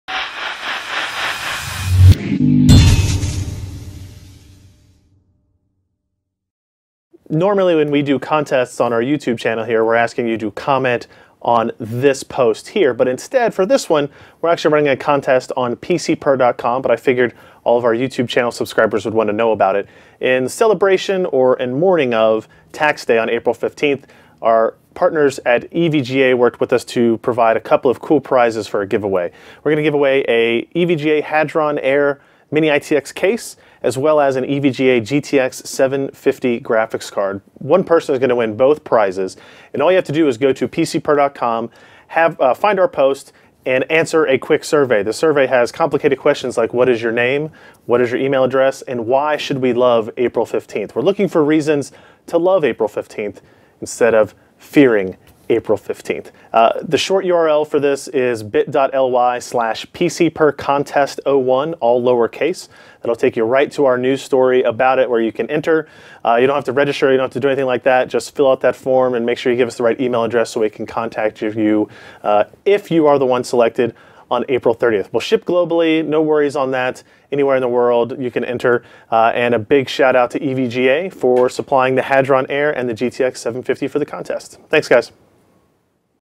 Post Tax Day Celebration! Win an EVGA Hadron Air and GeForce GTX 750!
In these good old United States of America, April 15th is a trying day. Circled on most of our calendars is the final deadline for paying up your bounty to Uncle Sam so we can continue to have things like freeway systems and universal Internet access.

But EVGA is here for us! Courtesy of our long time sponsor you can win a post-Tax Day prize pack that includes both an EVGA Hadron Air mini-ITX chassis (reviewed by us here) as well as an EVGA GeForce GTX 750 graphics card.